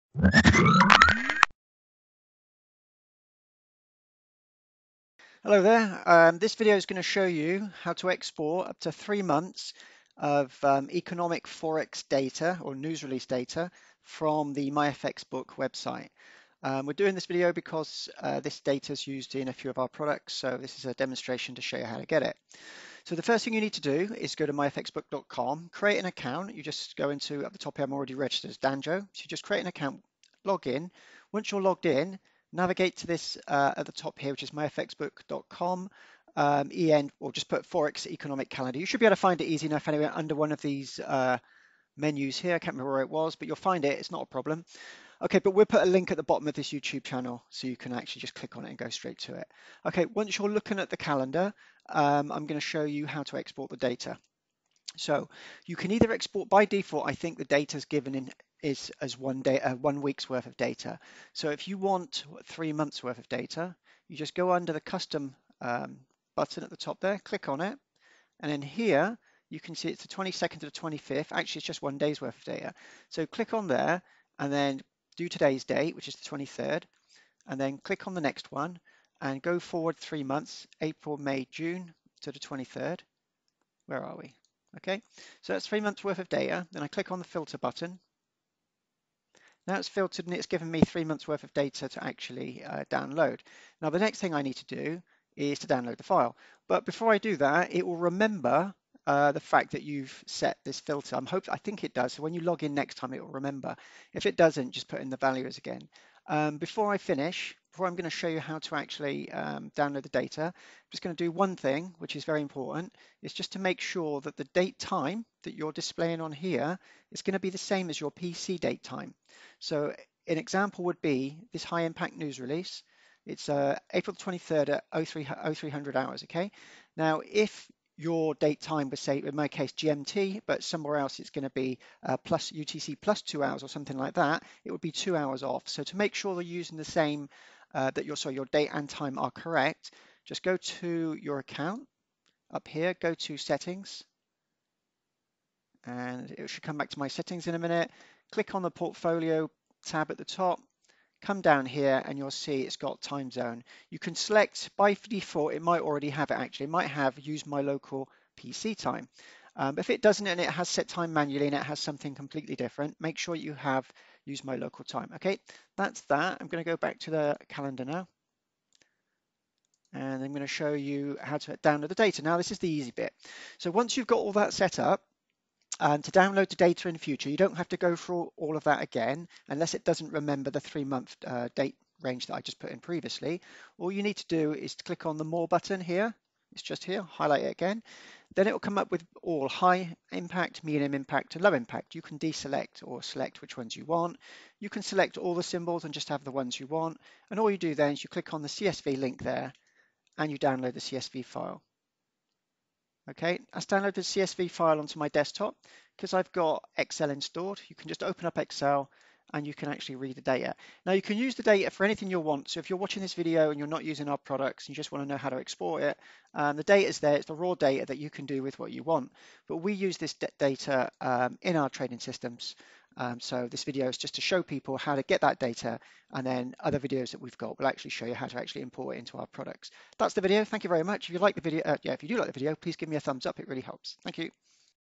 Myfxbook Forex News Calendar Export
This video will show you how to export up to 3-months worth of future economic news data from Myfxbook into a CSV file which can be read by any spreadsheet or even notepad. The core purpose of this video is to help our customers who will use this data in our financial tools.